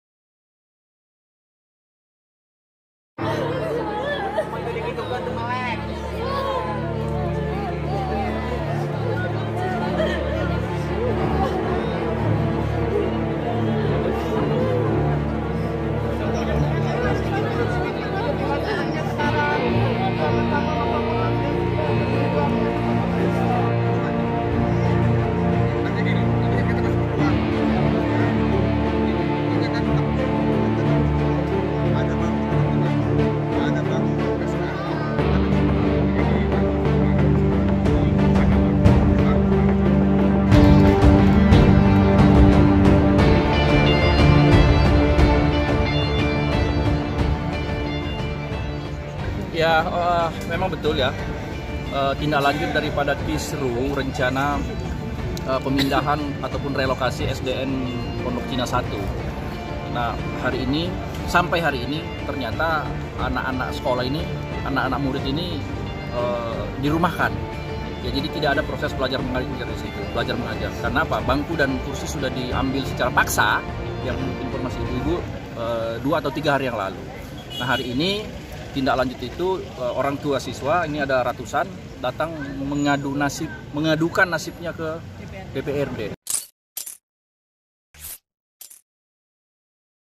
KARENA PEMBANGUNAN TROTOAR, RATUSAN ORTU MURID SDN POCIN 1 GERUDUK KANTOR DPRD DEPOK
Ratusan orang tua SDN Pondok Cina 1 Depok mendatangi kantor DPRD Kota Depok  dan mereka mempertanyakan nasib anak-anaknya yang hingga hari ini terlantar belajar karena Relokasi sekolah tersebut. Para orang tua siswa juga sempat menangis saat menyampaikan aspirasinya kepada anggota dewan.

Mereka menuntut agar Dinas Pendidikan Kota Depok bisa memfasilitasi para siswa untuk belajar.